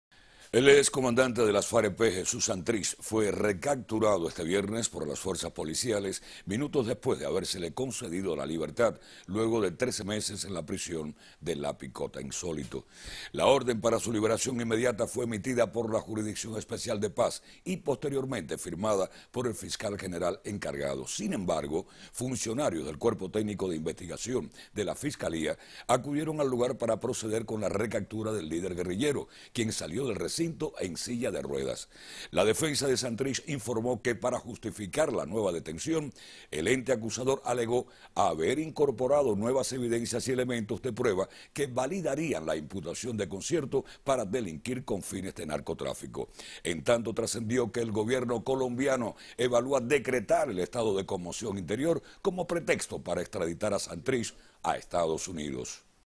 Liberan y recapturan a Jesús Santrich
El ex comandante de las FARC-EP, Jesús Santrich fue recapturado este viernes por las fuerzas policiales, minutos después de habérsele concedido la libertad, luego de 13 meses en la prisión de La Picota.